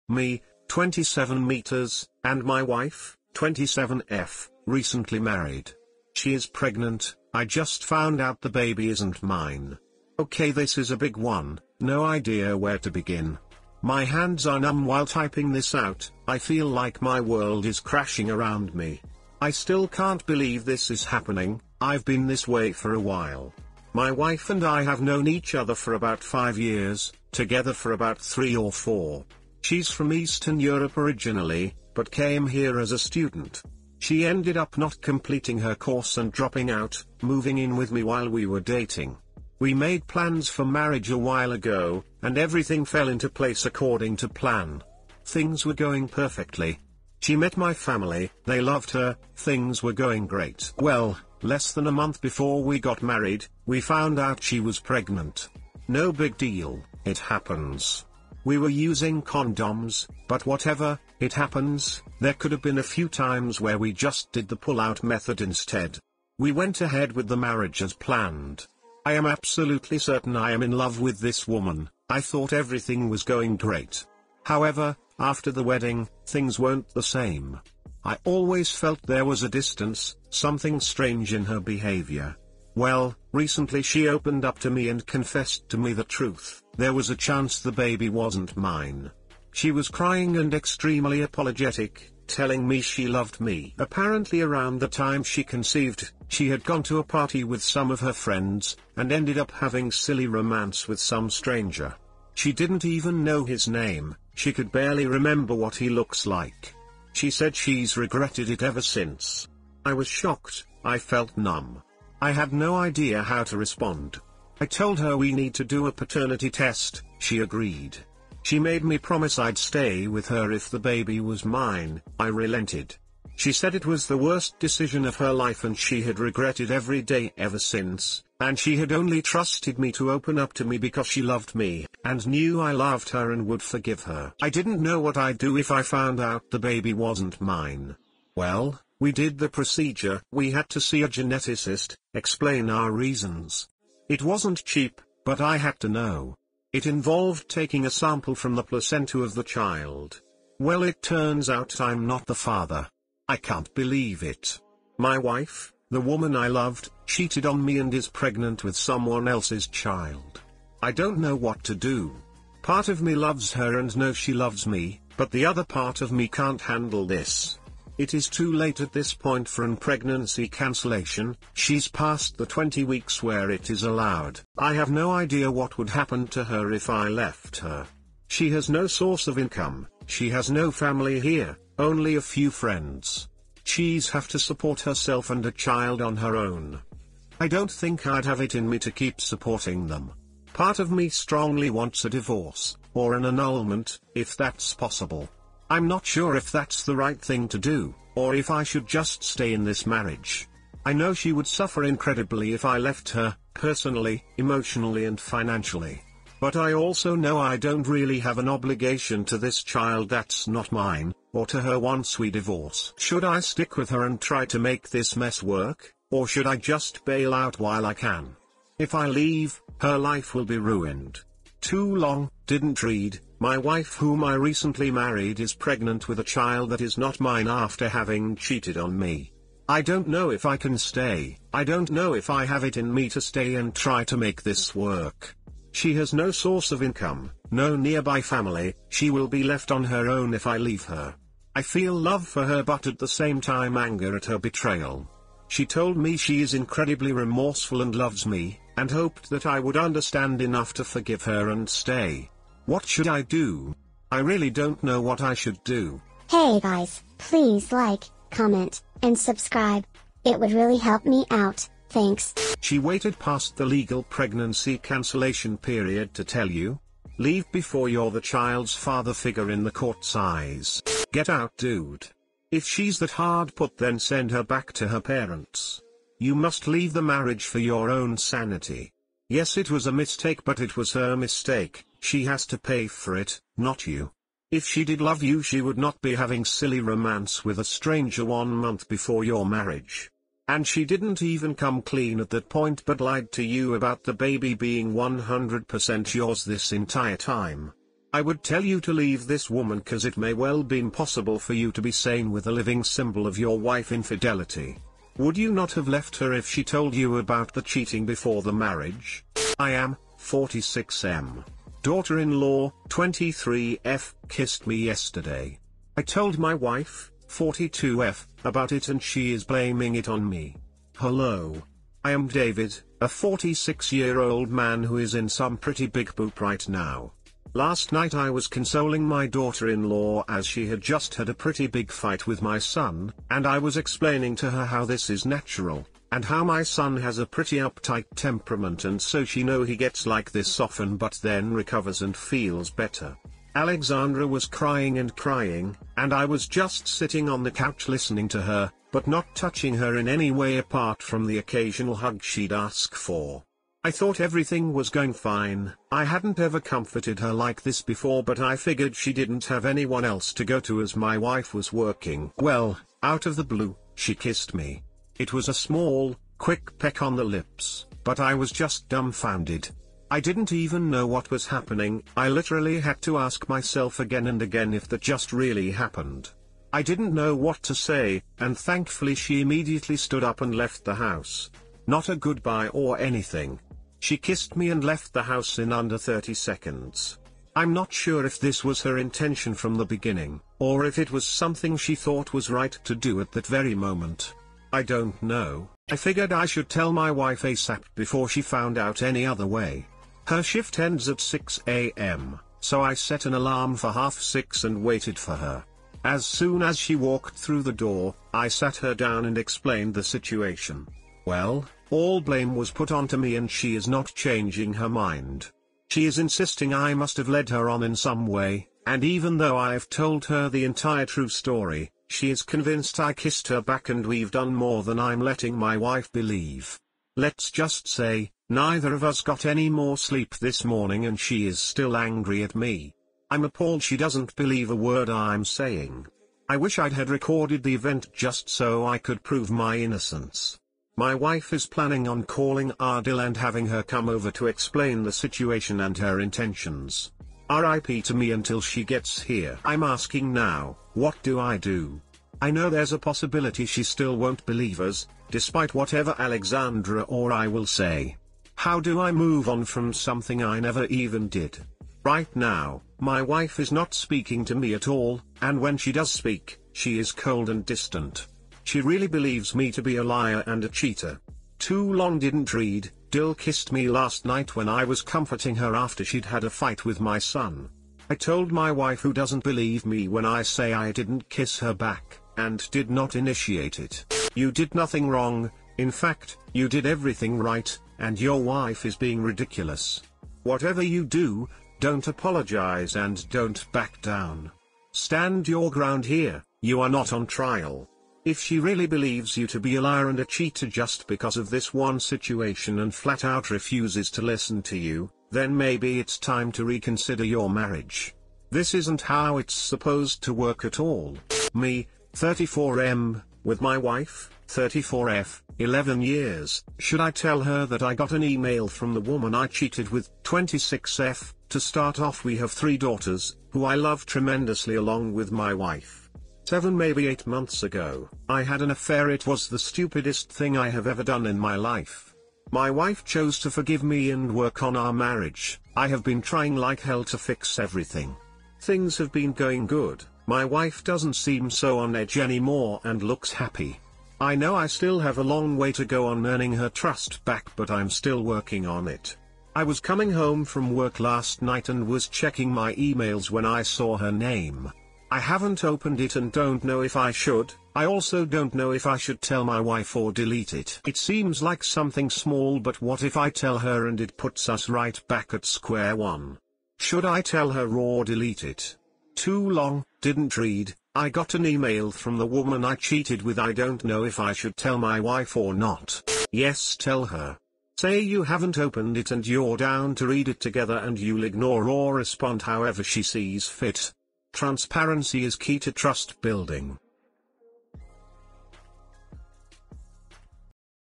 My Cheating Wife Is Pregnant, The Baby Isn't Mine.
Multiple reddit relationship stories about cheating. First story is about a newlywed couple where the wife cheated before they were even married, she got pregnant from the cheating and the guy just found out.

Apple MacBook Pro (13-inch)
Apple Macbook Air (13-inch)
Samsung Curved LED Monitor (27-inch)
Logitech G403 Wireless Gaming Mouse

Timestamps:
0:00 Me, [27m] and my wife [27f], recently married. She is pregnant, I just found out the baby isn't mine.
5:09 I am [46M]. Daughter-In-Law [23F] kissed me yesterday. I told my wife [42F] about it and she is blaming it on me.
8:31 Me [34 M] with my wife [34 F] 11 years, should i tell her that i got an email from the woman i cheated with [26 F].